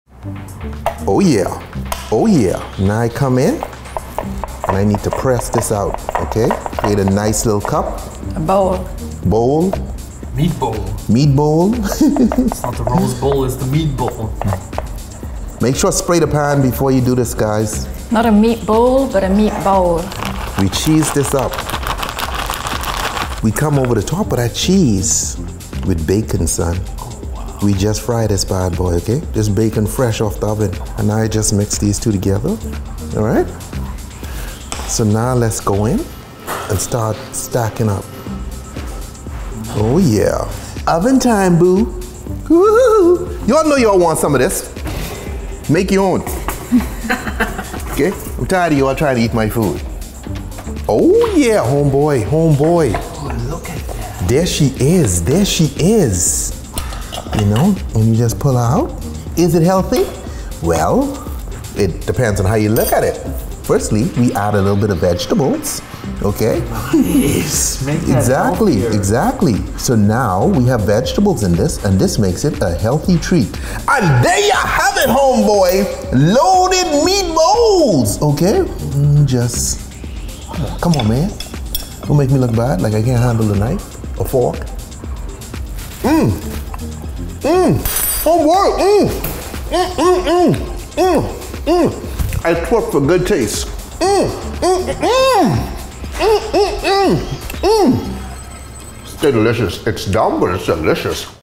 Loaded Cheeseburger Bowl
The bacon cheeseburger done muffin style 🍔